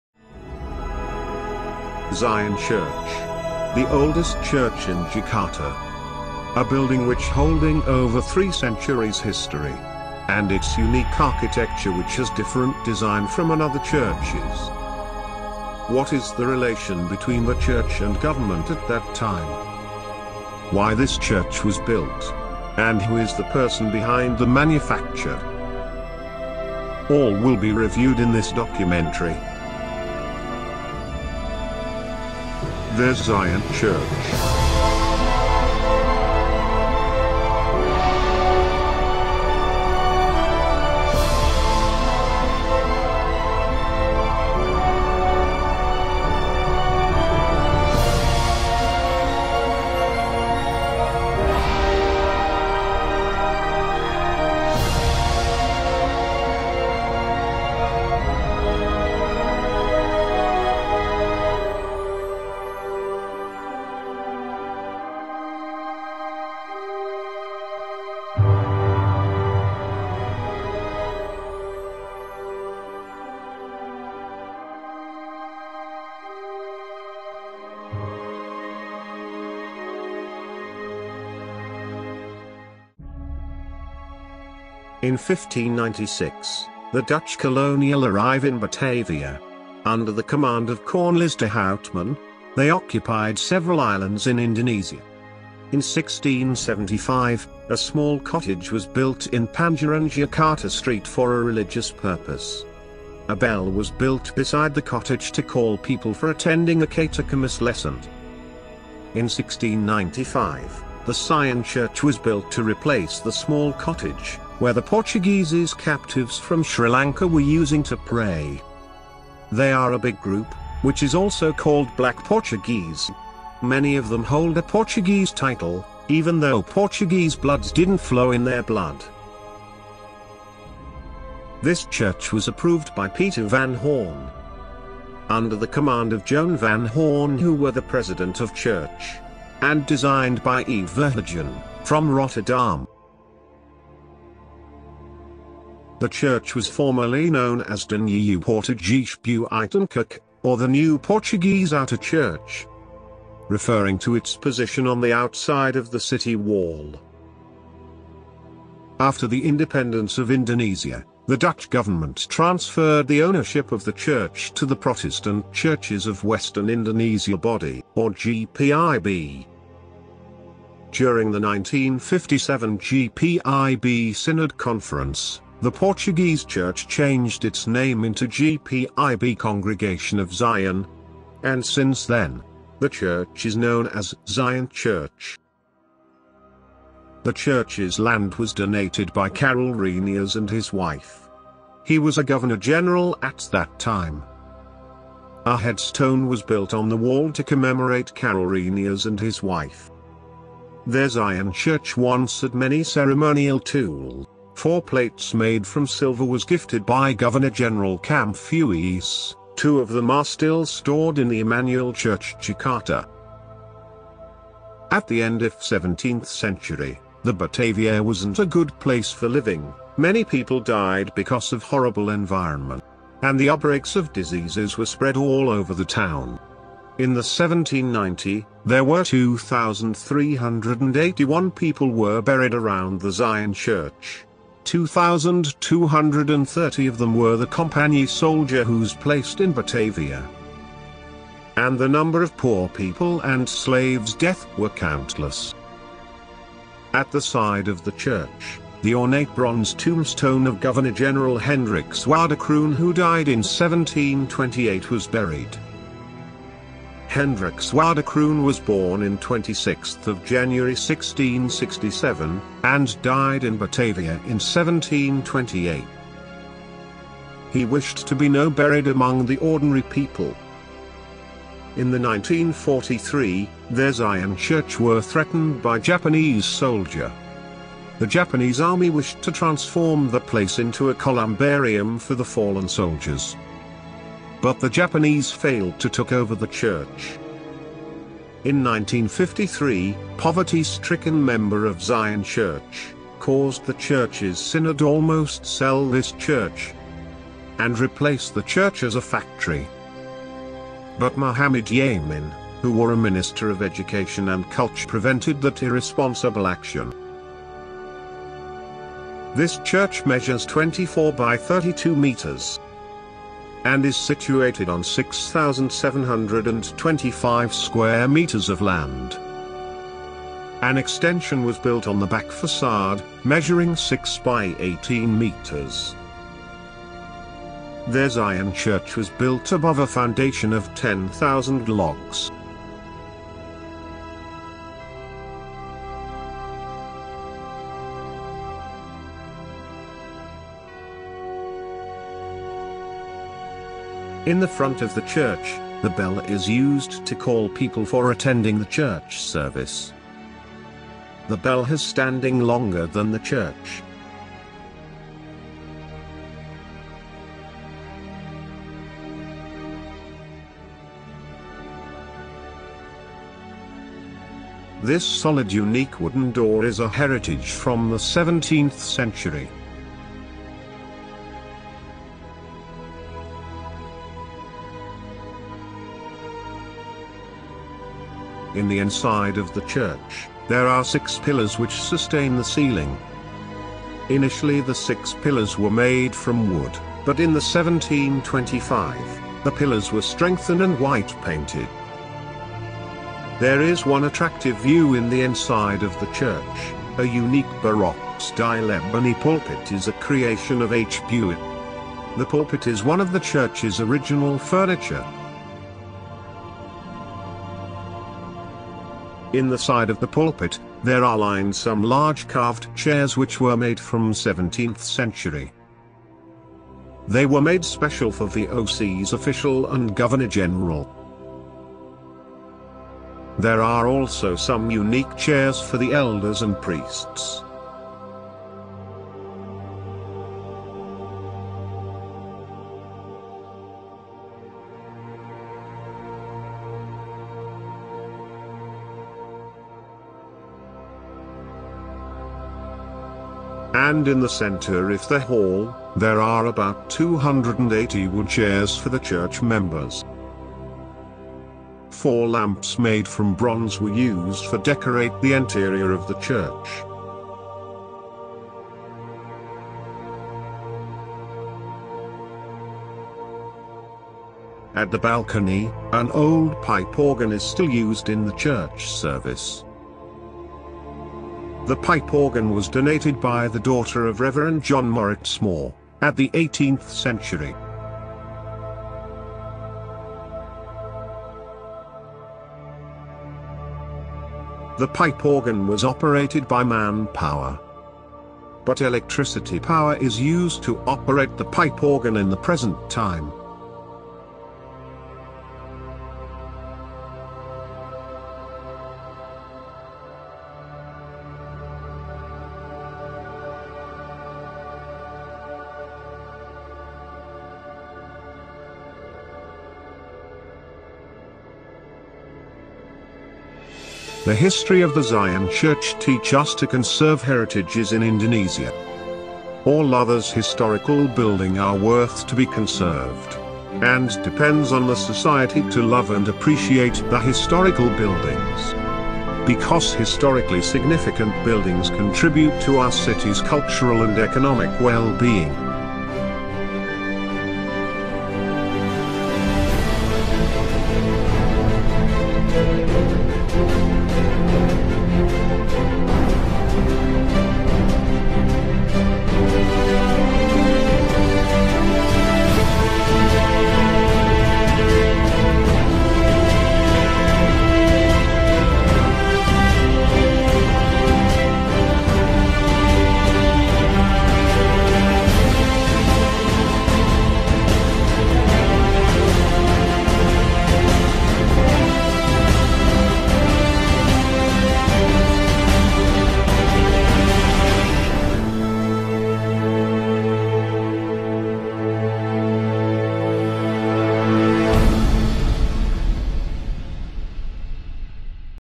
Film Dokumenter Gereja Sion/The Zion Church Jakarta (short documentary)
-NOT FOR COMMERCIAL USE- Dicky Kristianto-film dokumenter pendek Gereja Sion
directed by dicky kristianto